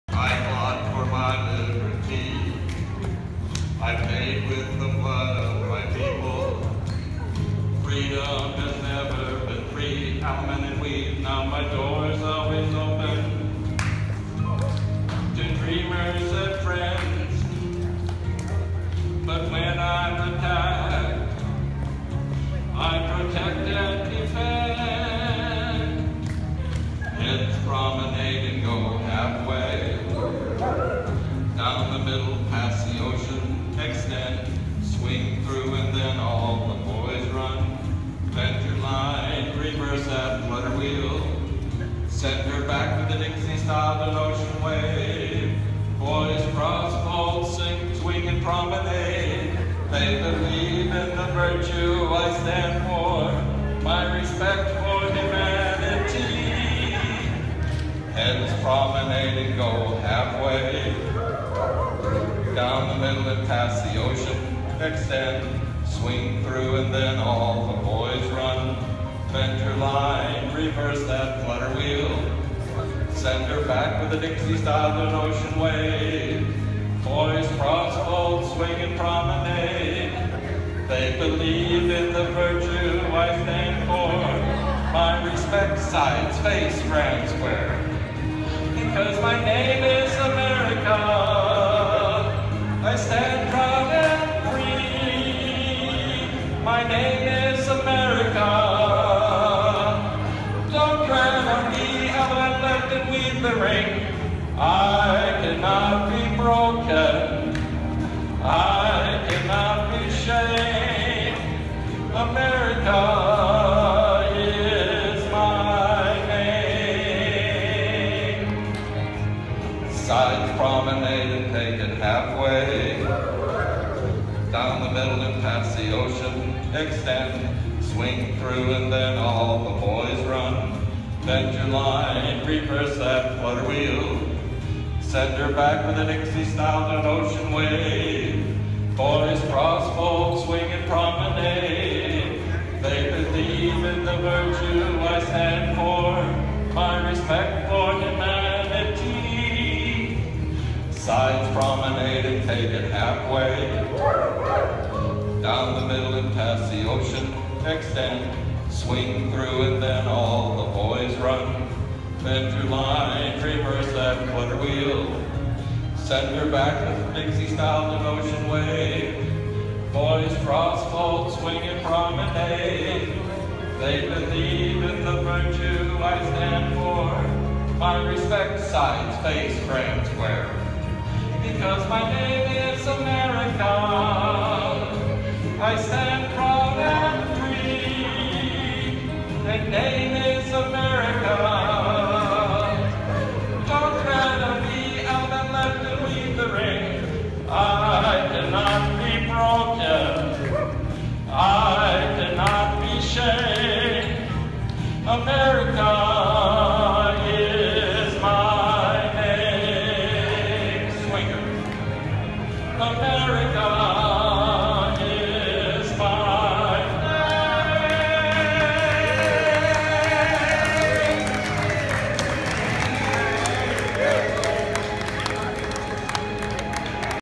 Swinging Saints Square Dance, Charlotte, NC July 9, 2016
July 9 2016 Graduation Dance
Caller David O Cox
Charlotte NC